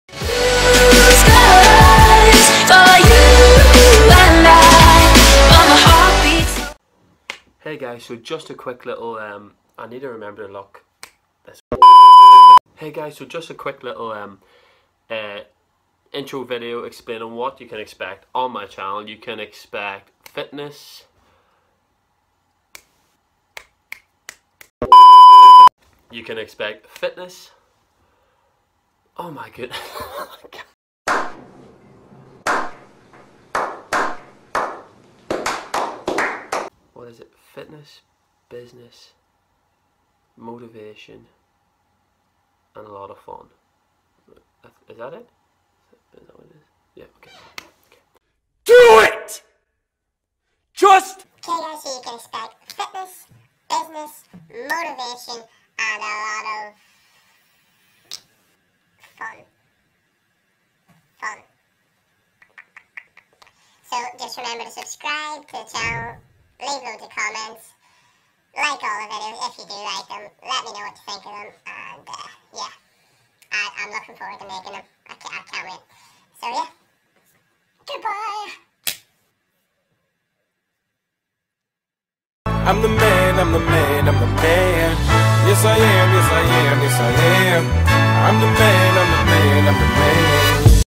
Welcome to my fitnessy, businessy, motivationy and funny channel!
Hello!

I have made this channel to hopefully bring a smile to peoples faces while sharing fitness tips, real life business experiences and motivation to keep going!

Tim

xo